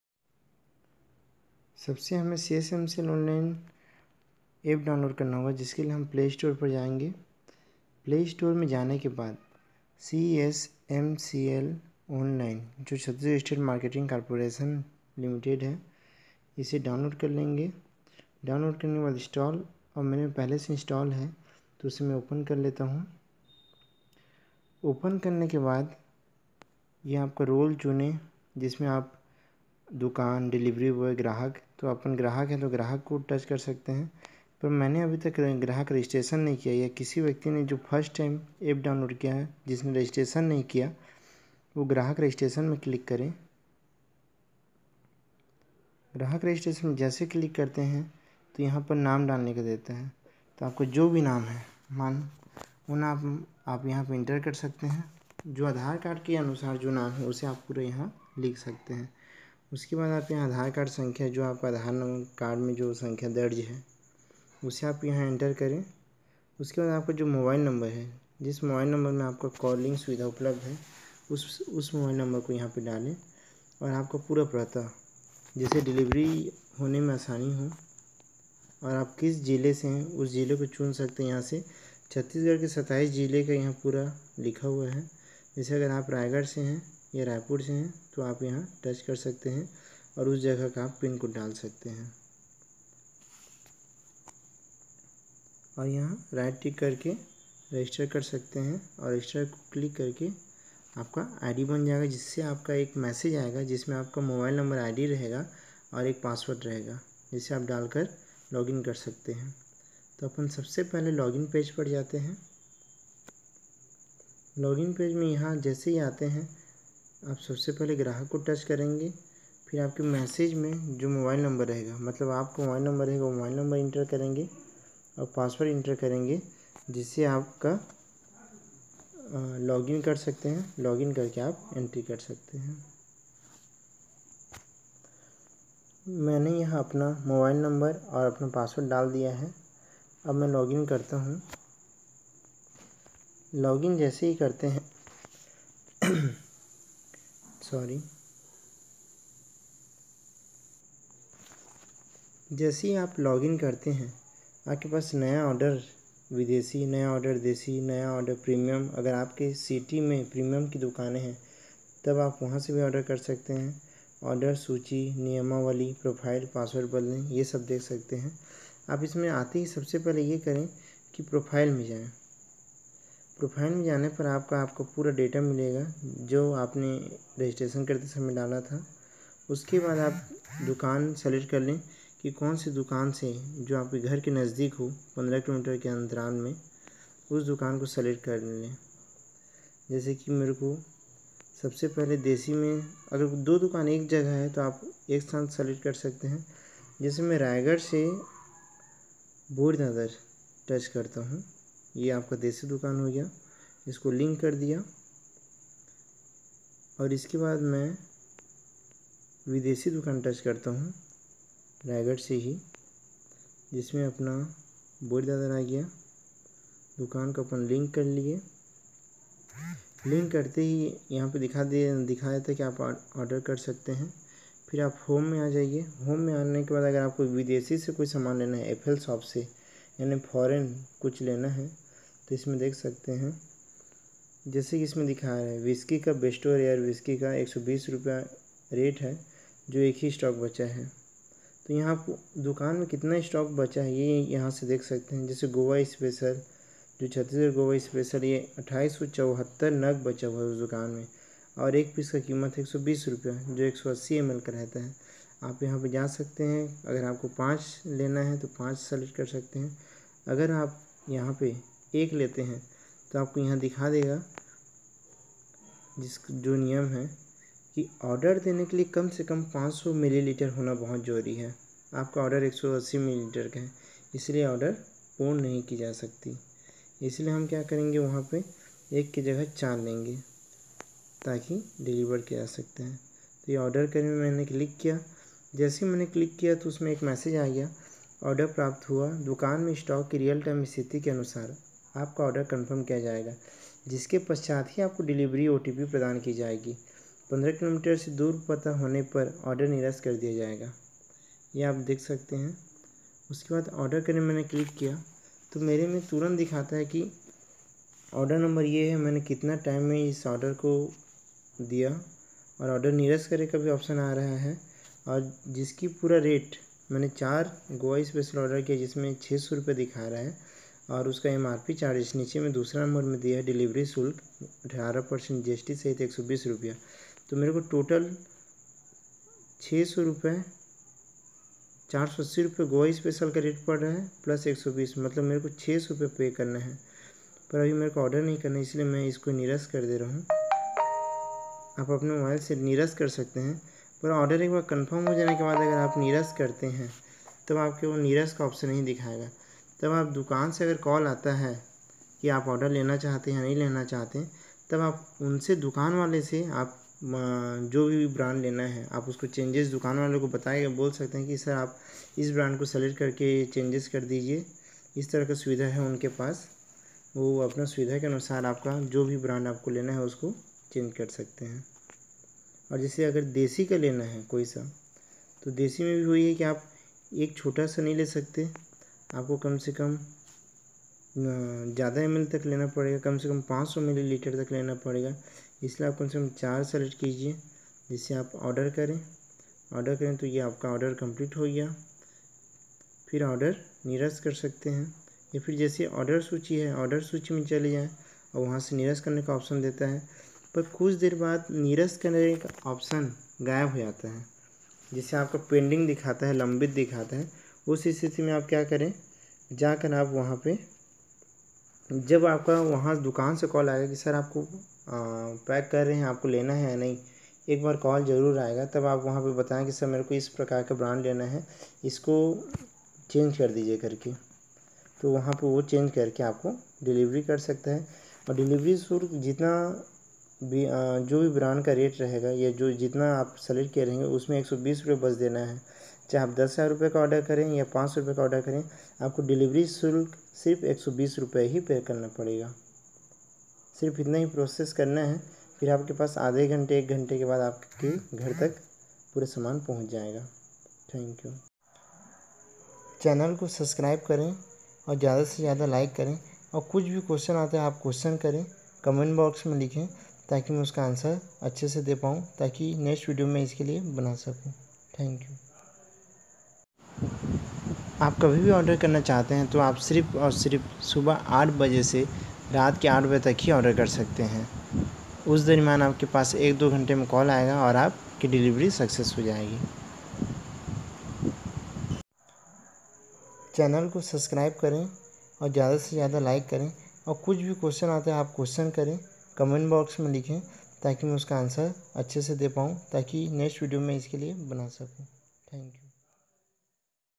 ऑनलाइन शराब कैसे मंगाए | online sharab kaise mangae | csmcl.in | दारू मंगाए।
Order kaise kare, registration kaise kare | website link - csmcl.in
Play store me search kare - csmcl online